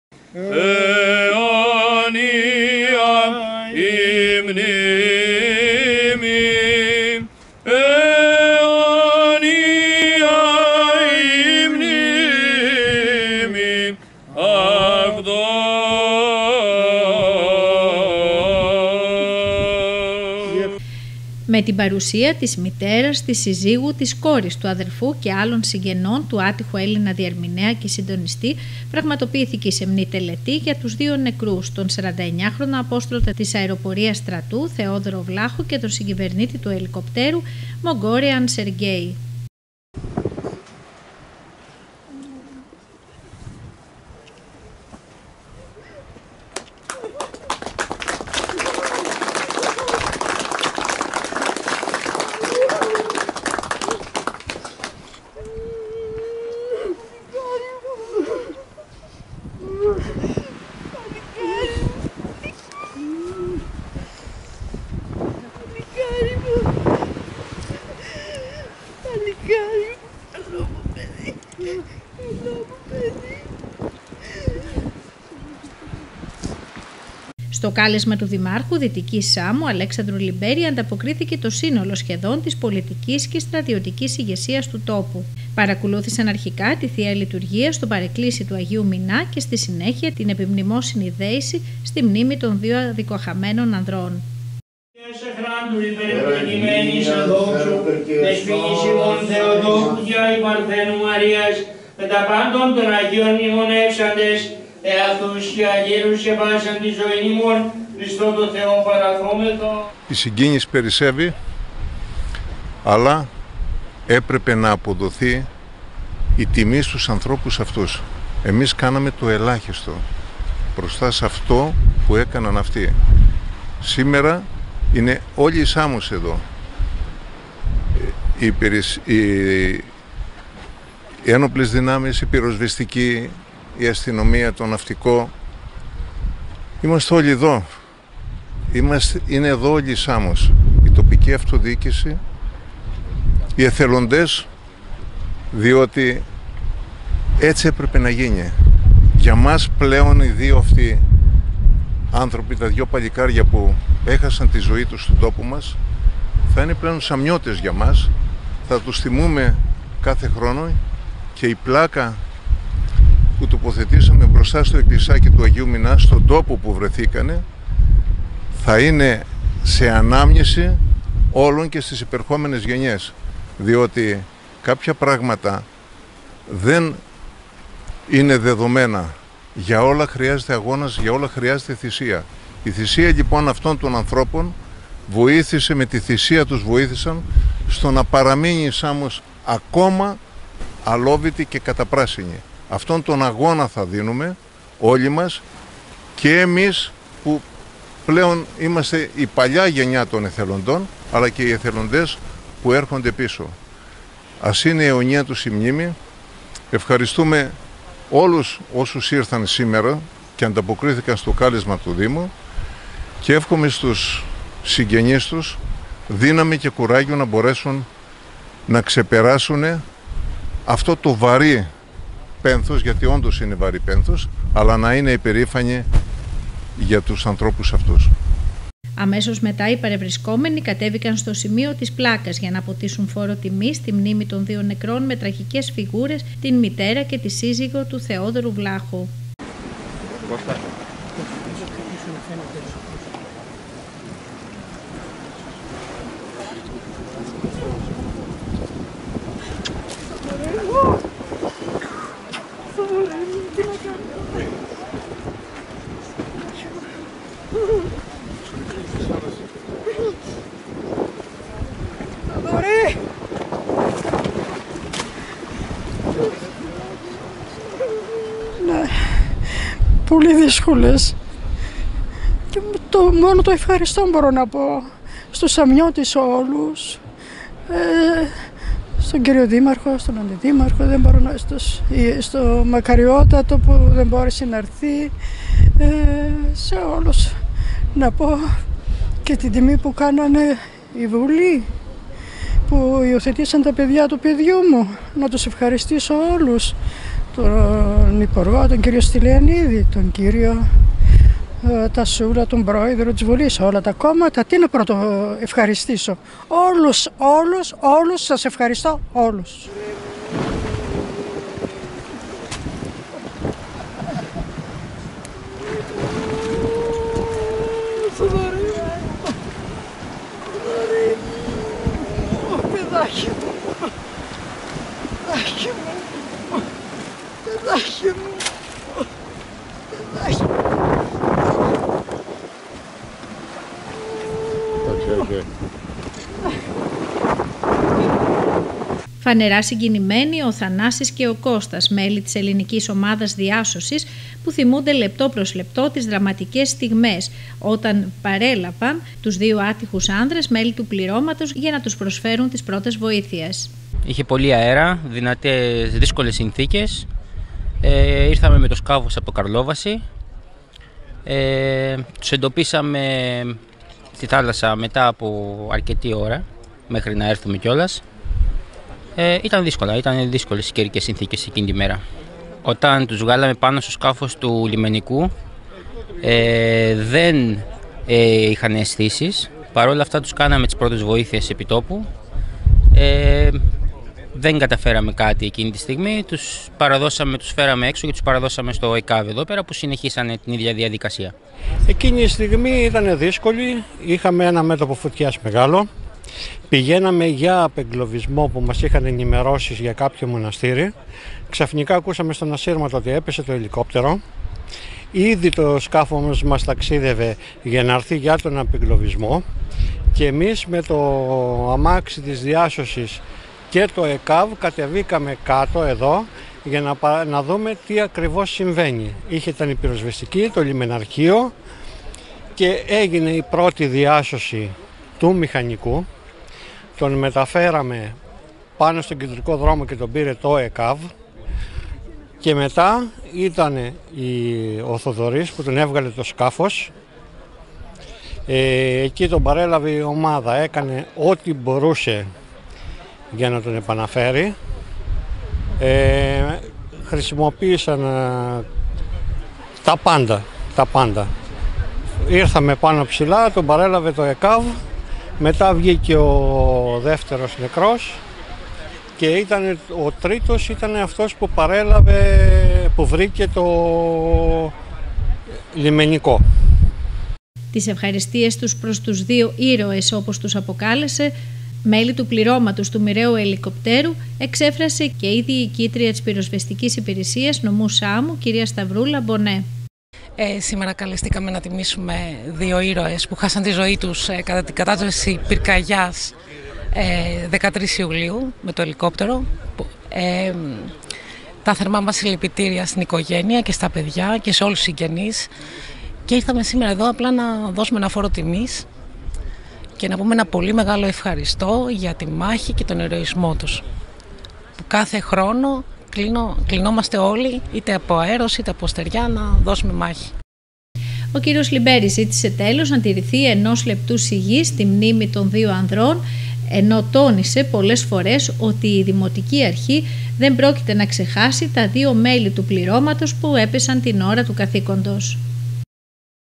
Αποκαλυπτήρια στην μνήμη των δύο ανδρών που έχασαν τη ζωή τους στην πτώση του ελικοπτέρου στη Σάμο
Σε κλίμα μεγάλης συγκίνησης ο Δήμος Δημοτικής Σάμου τίμησε την μνήμη των δυο μελών του πυροσβεστικού ελικοπτέρου που κατέπεσε στην θαλάσσια περιοχή της Πλάκας.
Το ελικόπτερο με το τετραμελές πλήρωμα απογειώθηκε από το αεροδρόμιο της Σάμου στις 16.39  της 13ης Ιουλίου και κατέπεσε στις 17.55 στη θαλάσσια περιοχή Αγιος Μηνάς, κοντά στην παραλία Πλάκας, νοτιοδυτικά του νησιού.
Σε αυτό επέβαιναν ένας Ρουμάνος, δύο Μολδαβοί και ένας Ελληνας διερμηνέας, ο οποίος είχε ρόλο συνδέσμου για να συντονίζει το έργο του ελικοπτέρου με τις επίγειες δυνάμεις που επιχειρούν στο μέτωπο της πυρκαγιάς.
Ο Έλληνας απόστρατος της Αεροπορίας Στρατού, Θεόδωρος Βλάχος θα χάσει την ζωή του, οπως και ο Ρουμάνος Μηχανικός.
Ο 49χρονος, πατέρας δύο ανήλικων παιδιών, θα έπαιρνε άδεια από τις 31 Ιουλίου για να ξεκινήσει τις καλοκαιρινές διακοπές με την οικογένειά του.